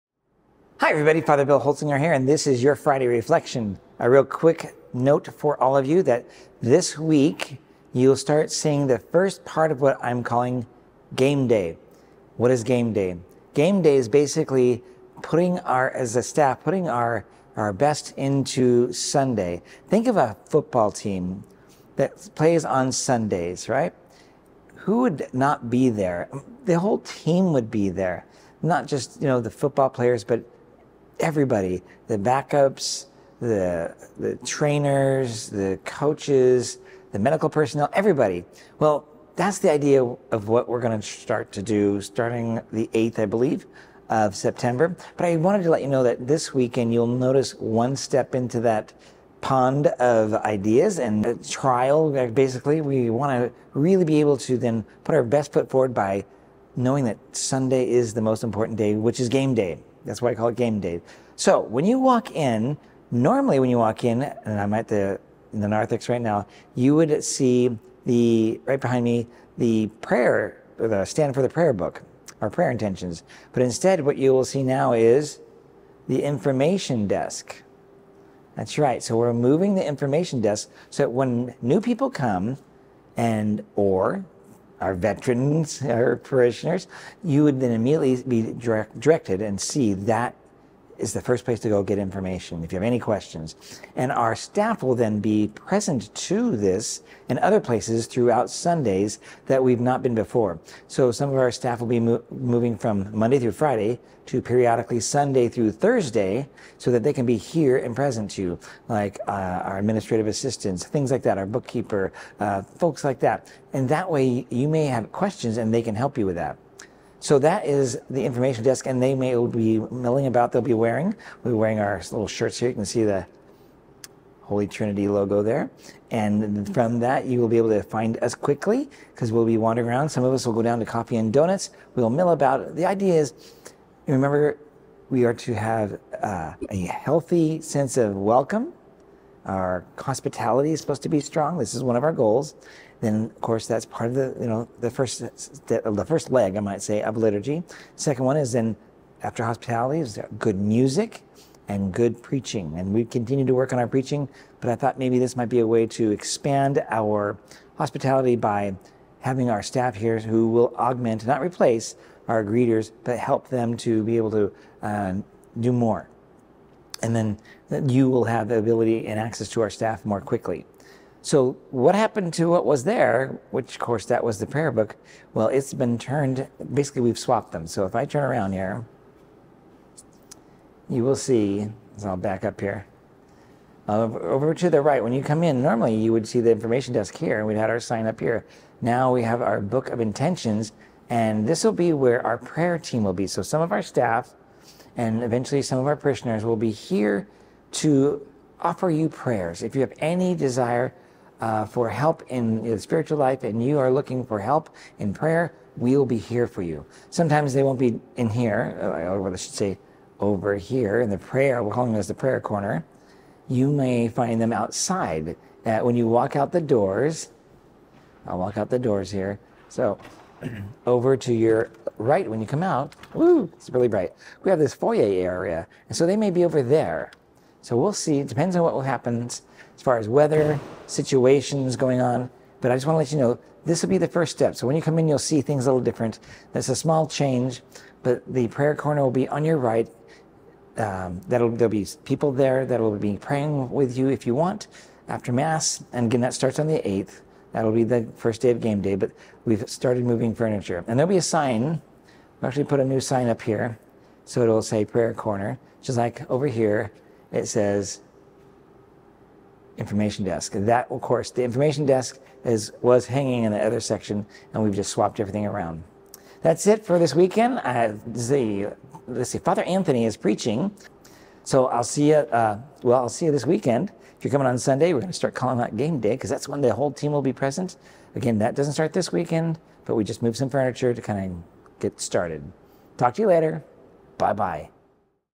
Friday Reflection: "Game Day Pt. 1"
Join Fr. Bill as he discusses a new concept for improving our mission for Sunday Mass: GAMEDAY!